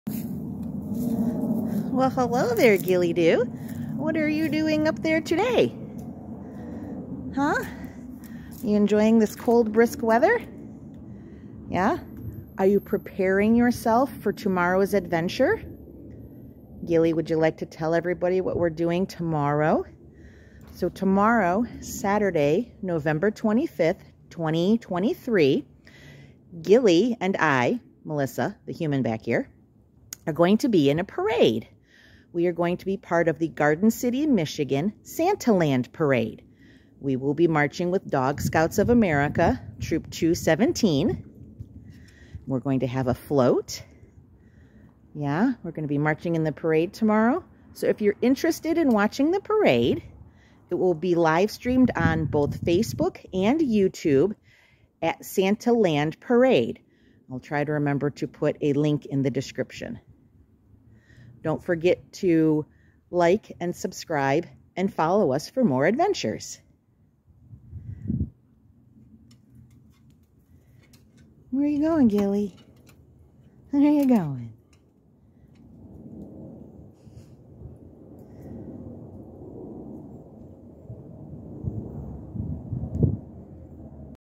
Who Loves a Parade?
Ghillie Dhu and Melissa will be in the Santaland Parade in Garden City, Mi on Sat 11/25/23 starting at 10am.